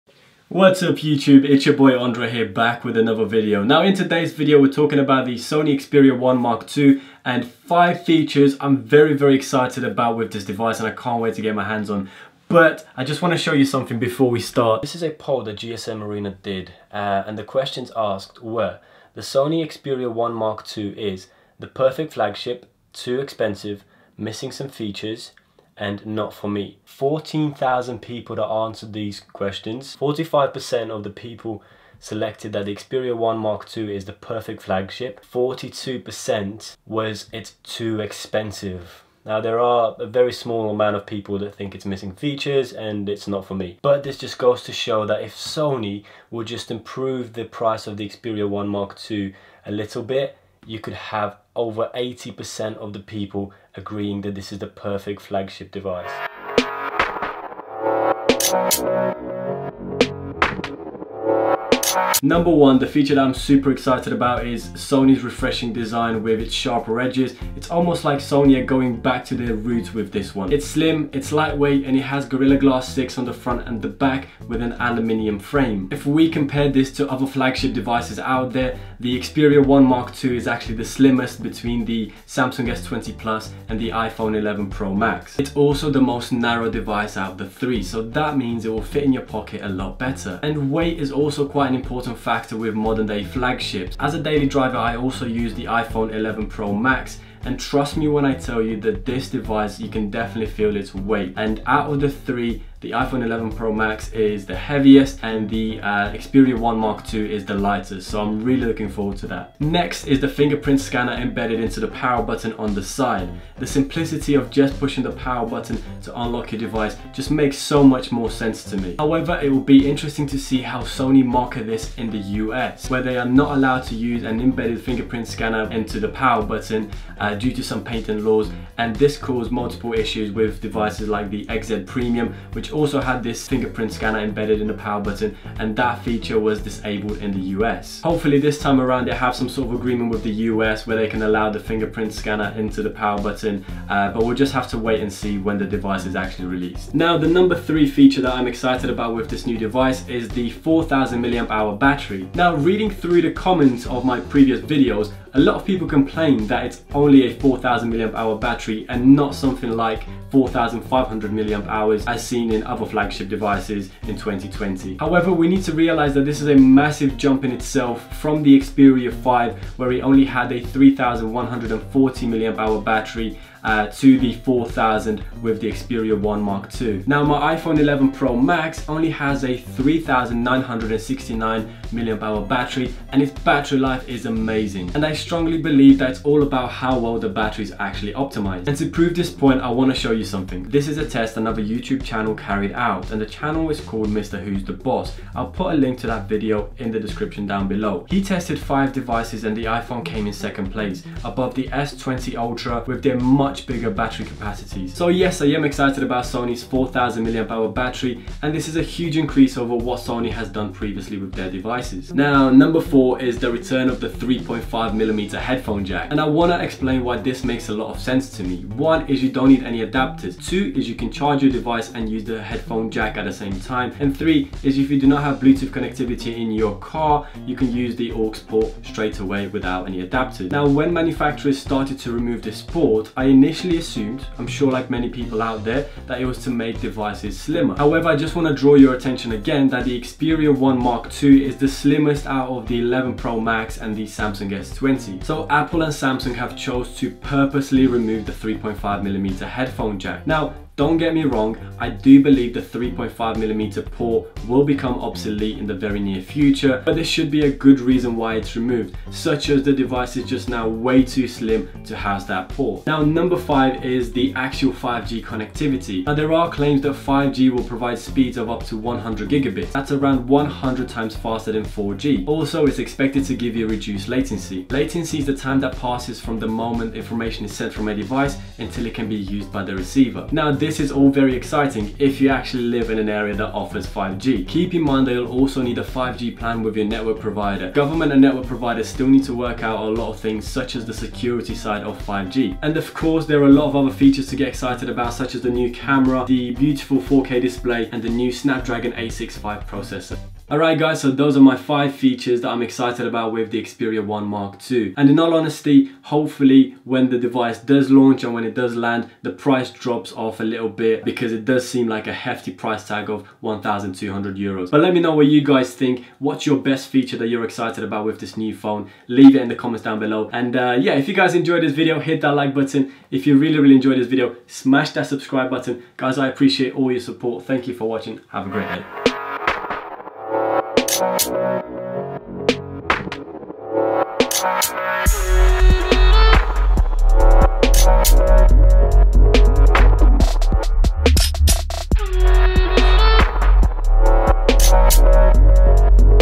Sony Xperia 1 II 2020 | 5 Amazing Features
Looking at 5 Amazing Features of the Sony Xperia 1 Mark 2.